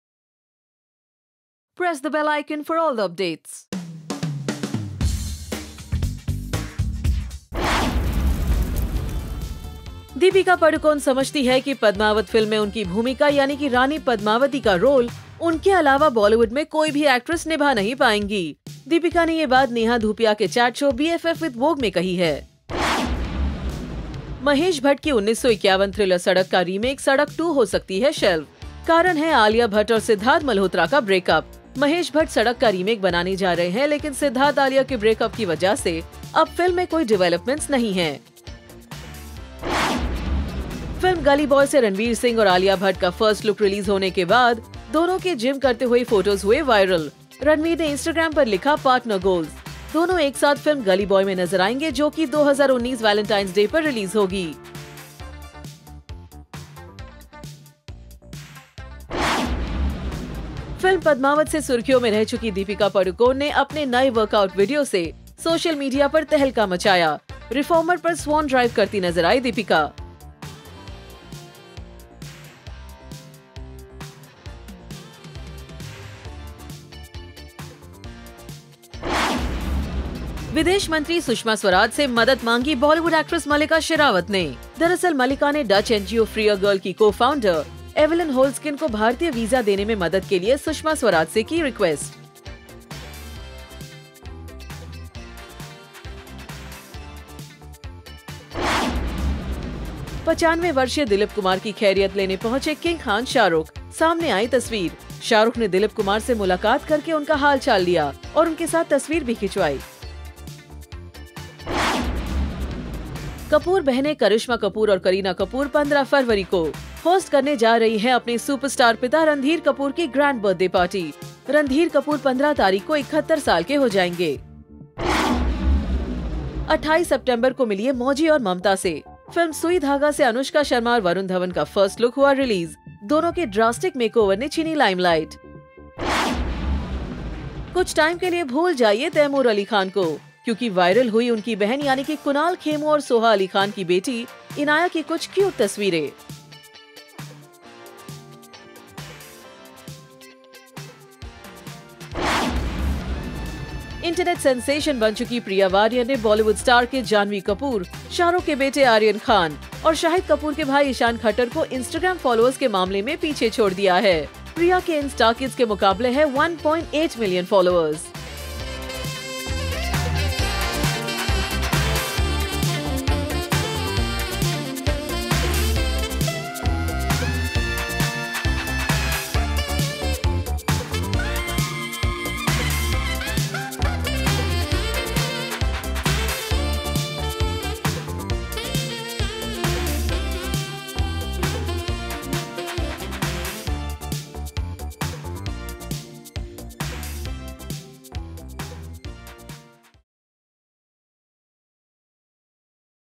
Priya Prakash Varrier, Anushka Varun Sui Dhaaga, Inaaya, Shah Rukh Visits Dilip Kumar | Daily Wrap
Catch all the latest Bollywood gossips, from the internet sensation Priya to Anushka Varun's new look only on Daily Wrap.
Report by: Abhishek Halder
Produced by: Triparna Rout
Editor: Rupesh Shinde